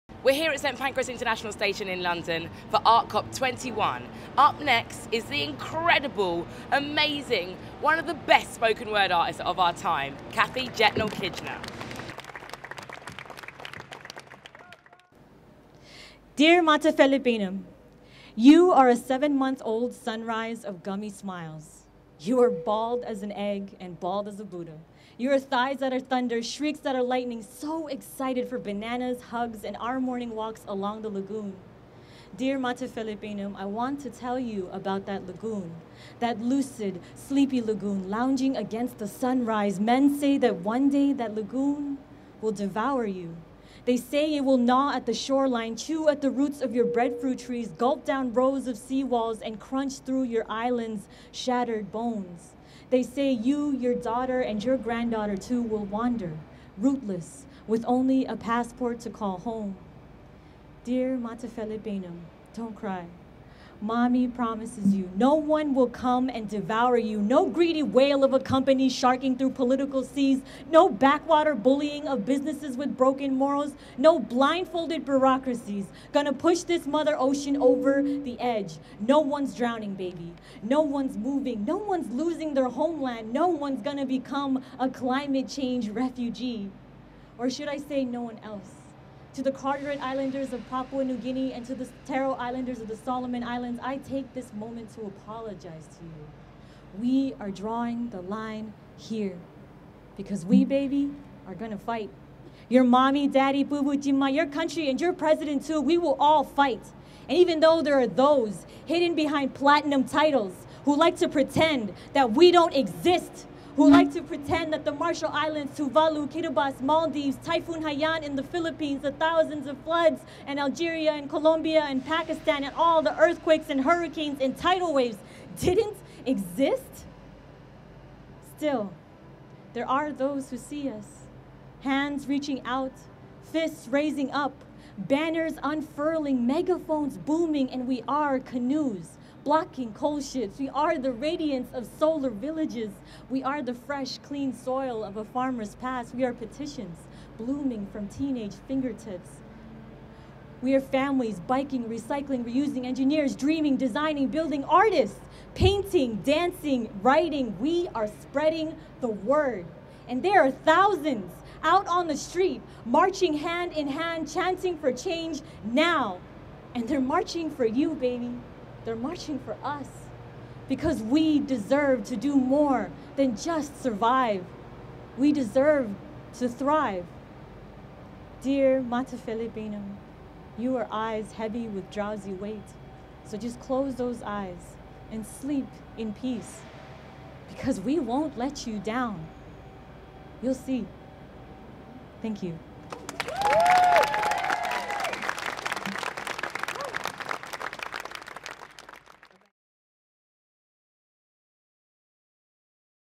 UN Climate Poet Kathy Jetnil-Kijiner performs Dear Matafele Peinem'
ArtCop21 and Good Energy staged a live and loud cultural takeover of St. Pancras International Station as the UN Climate Change talks began.